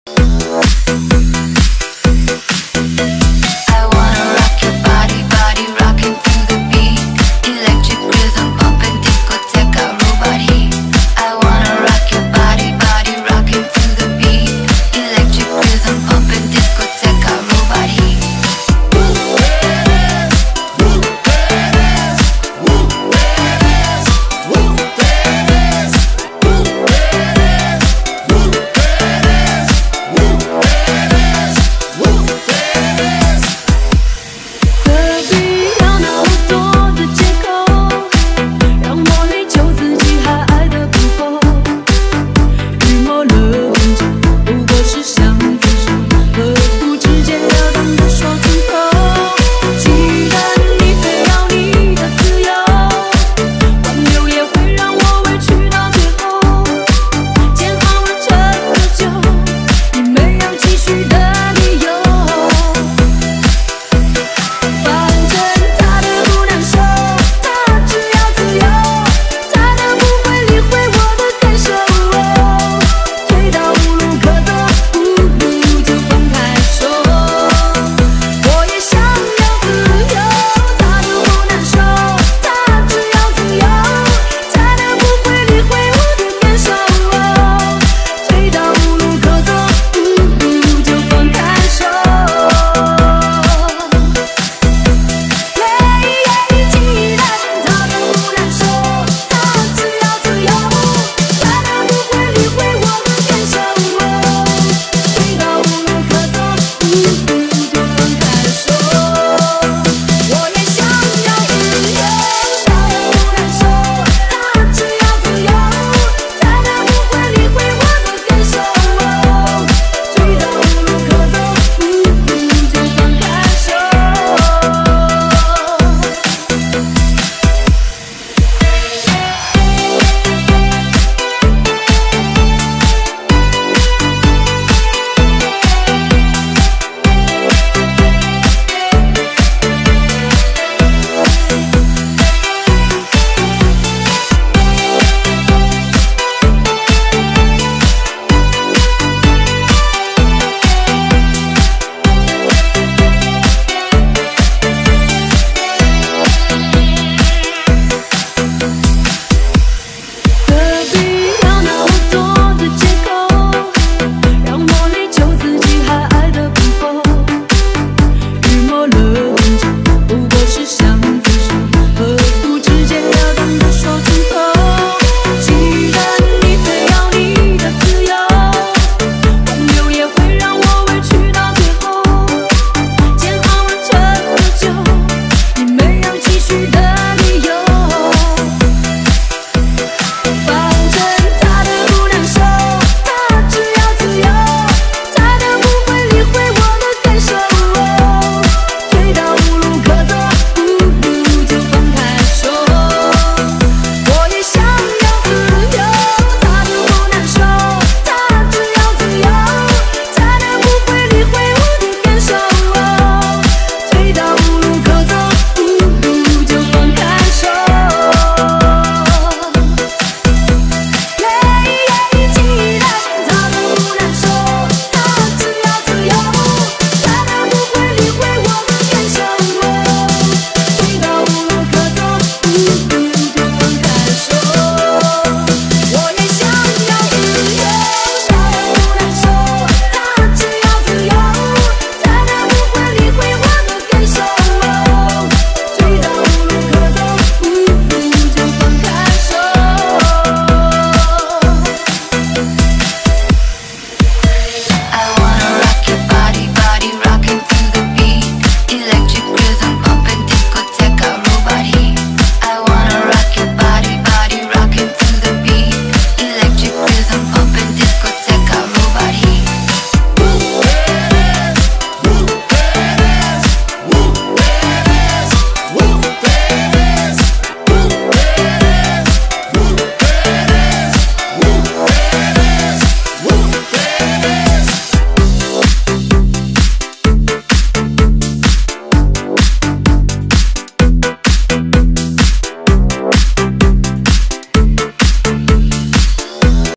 BMW X4 輪胎更換德國馬牌225/60-17 LX SPORT
BMW X4 輪胎更換
德國馬牌225/60-17 LX SPORT
🇮🇹義大利 MF AQUILA TORNADO 免翹棒拆胎機安裝
🇮🇹義大利 MF MT3900up C 全自動偵測平衡機校正
🇮🇹義大利 MF 八鏡頭3D四輪定位

✅魔法藥水鋁圈清洗
✅去膠輪機殘膠去除
✅鋁圈紅外線真圓偵測
✅輪胎胎面聲納偵測

💳國民旅遊卡💳信用卡💰台灣Pay……皆可結帳

⚠️服務地址：員林市條和街258號